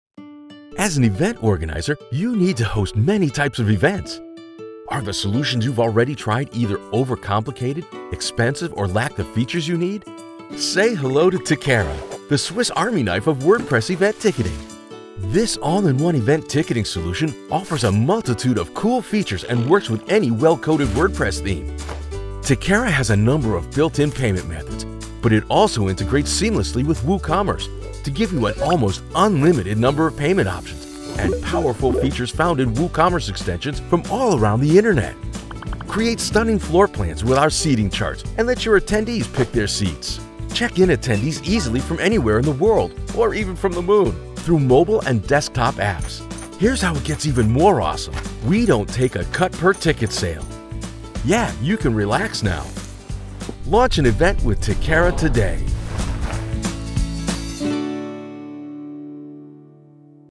Tickera - Explainer Video (3D Animated)
Tickera is a WordPress event ticketing system that allows you to sell event tickets on your website and deliver to buyers digitally, without having to give any cut to third parties. There are more than 20 built-in payment gateways plus all WooCommerce gateways.

With Tickera's smartphone apps for iOS and Android. you can check in attendees anywhere at any time of the day. Attendees can pick the best seat through the stunning floor plans of the event venue.